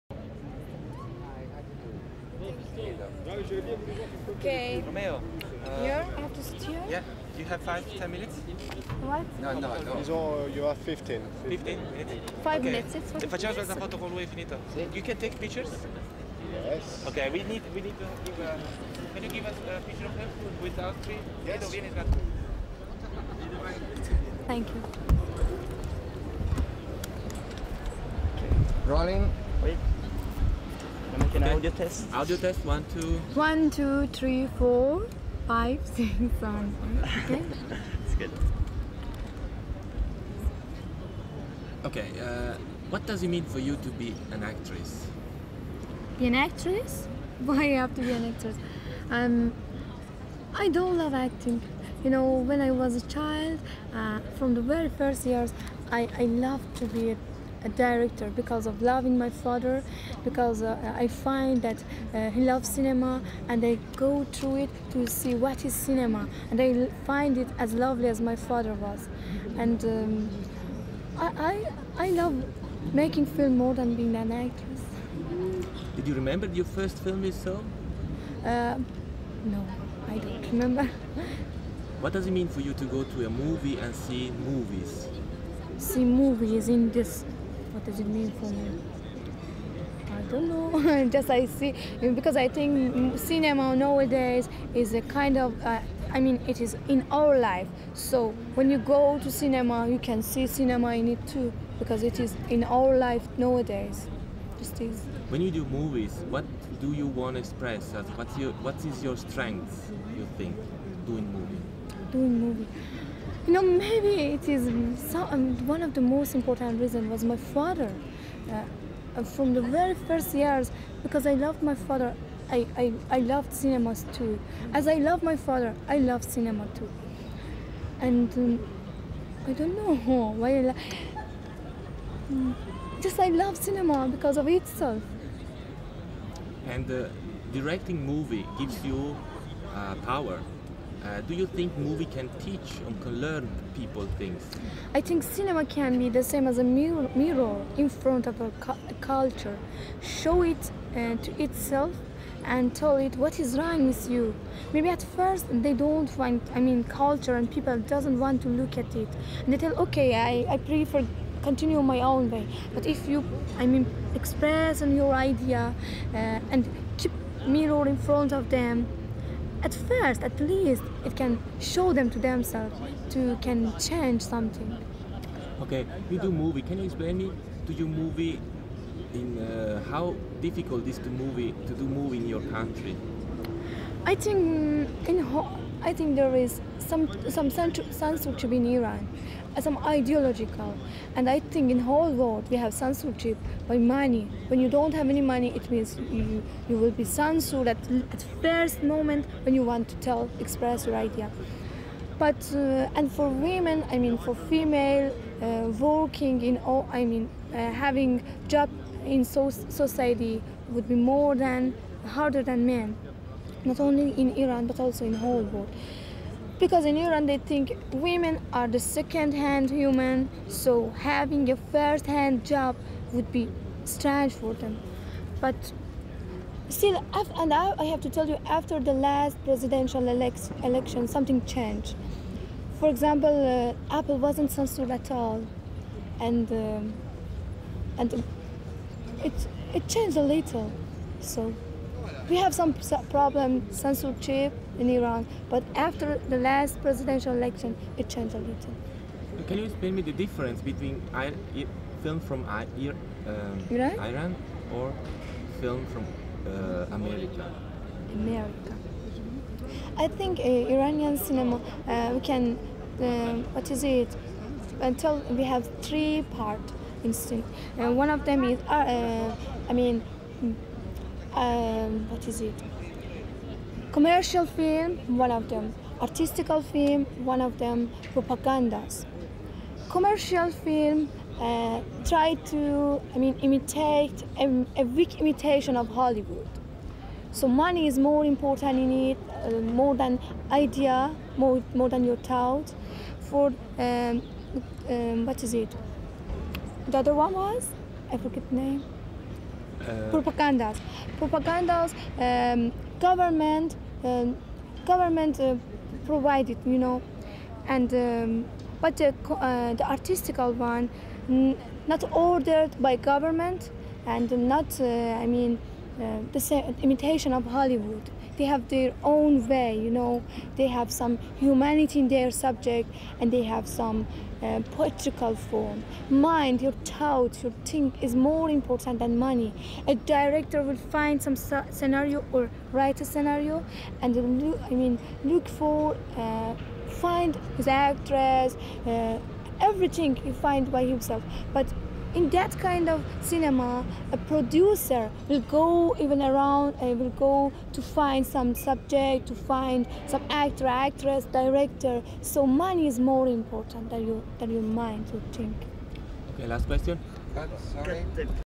Samira Makhmalbaf, as I love my father I love cinema too
Samira Makhmalbaf, born February 15, 1980, is an internationally acclaimed Iranian filmmaker and script writer. Samira Makhmalbaf is considered to be one of the most influential directors as part of the Iranian New Wave.

Samira Makhmalbaf si racconta a Cannes nel 1998, intervistata da Alberto Veronese (regista di quello che poi è diventato il documentario Homo Cinematographicus), Edo Bertoglio e Vito Robbiani.